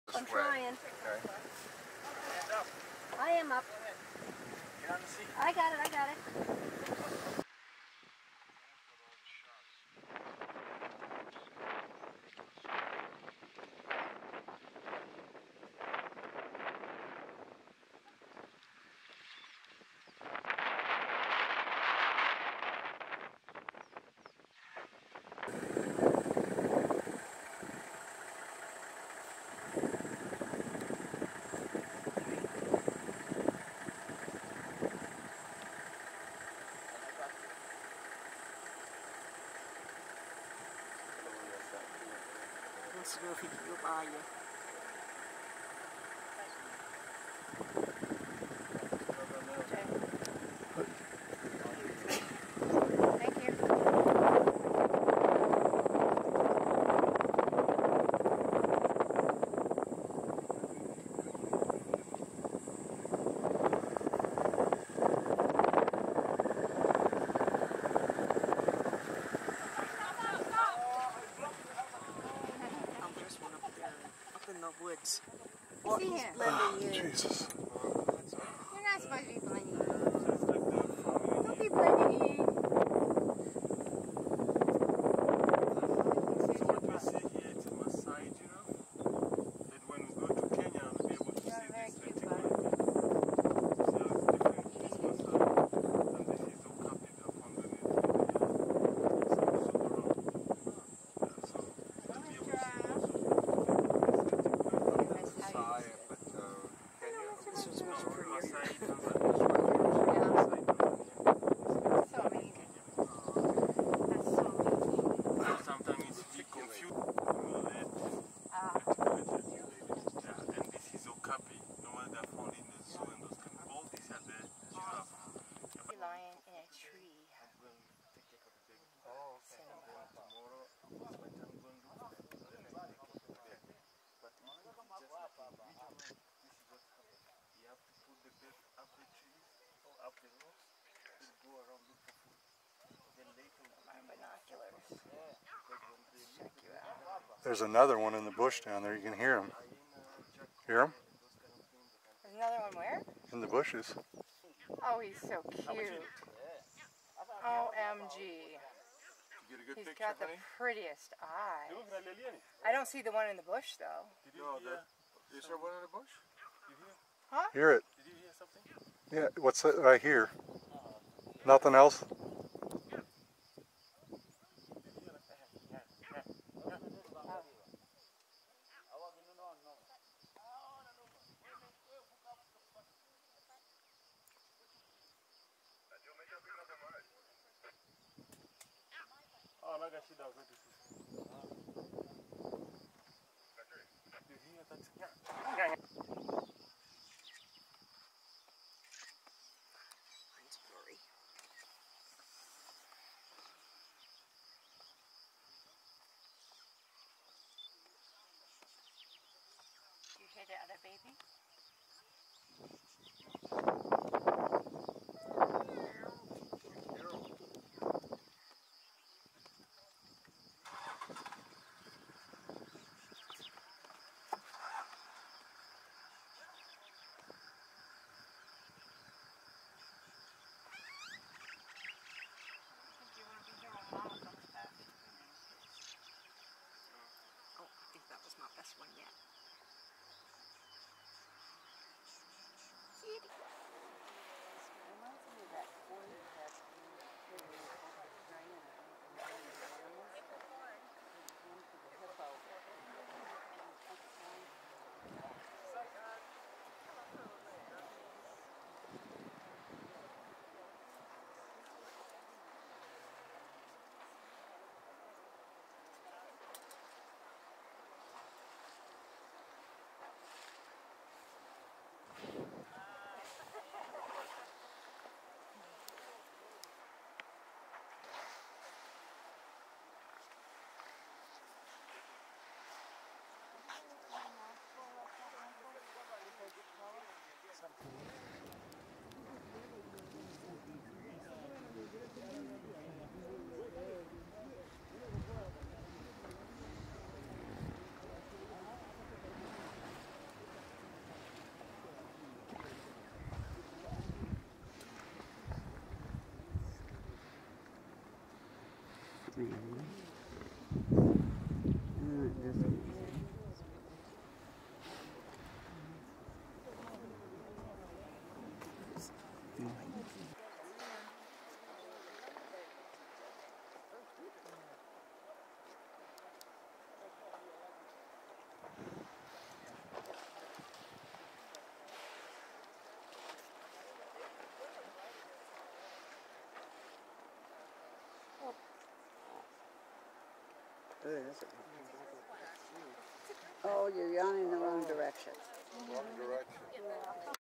2020 Africa Trip - Serengeti Glimpse
Some video footage of wildlife shot in the Serengeti, highlights of which is a very cute lion cub hanging out in a tree waiting for his mom to return.